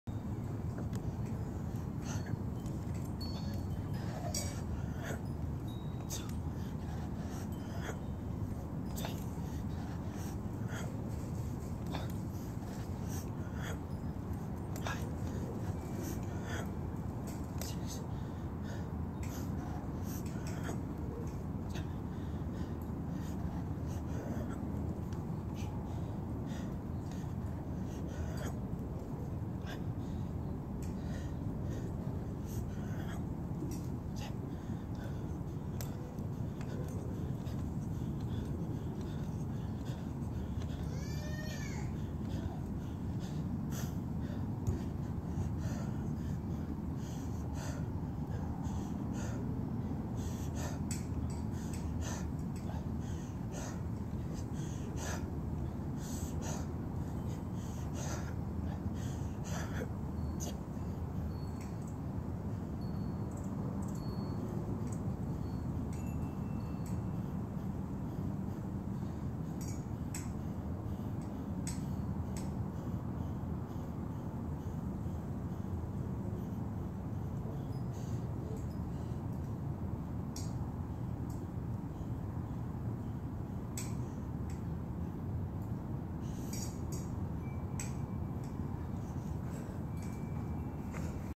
10 clean muscle ups to 10 bar dips to 10 pull ups in 58 seconds plus isometric holds!
Cool down set for dinner!